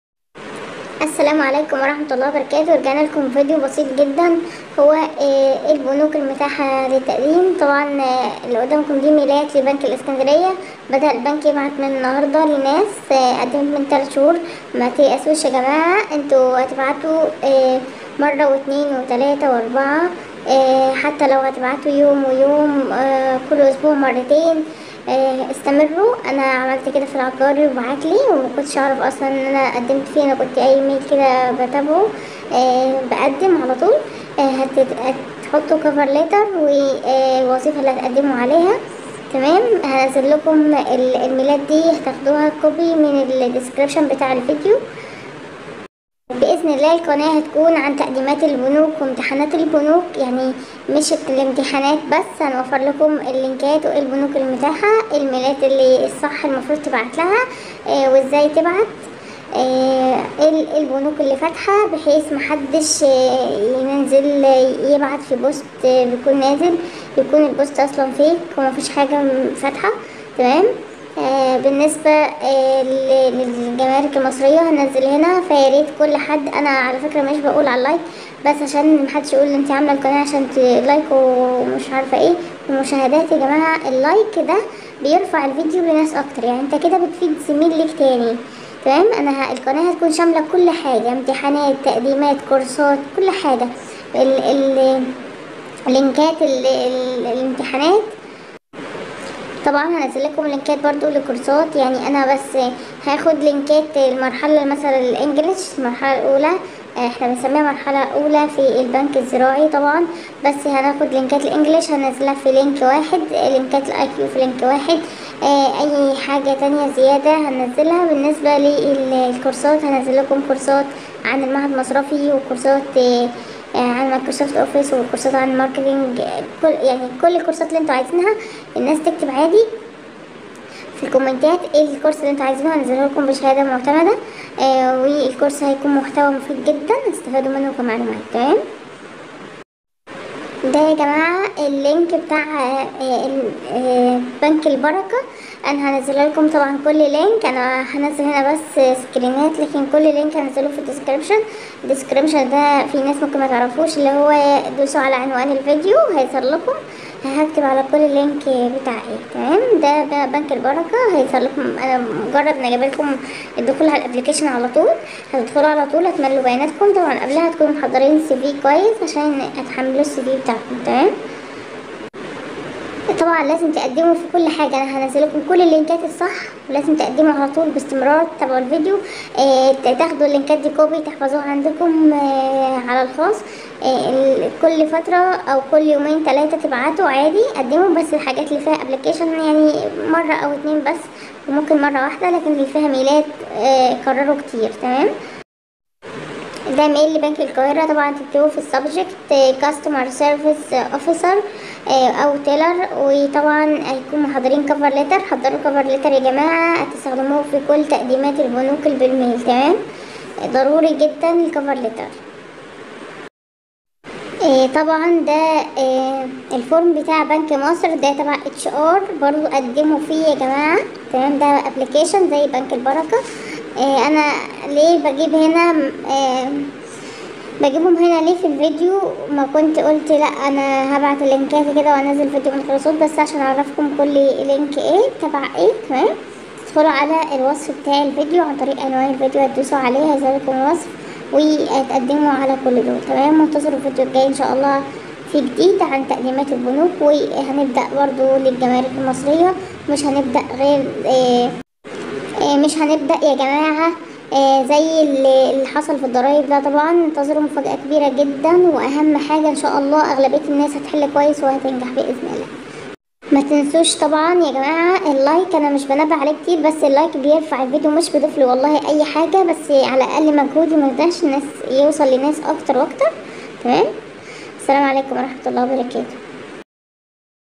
اخر بنوك فاتحه تقديم وانتظروا تدريب iq بنك اهلى وقاهره لانه اصعب iq هتقابلوه👌
فورم بنك مصر

ميلات بنك اسكندريه وجه قبلى

بنك البركه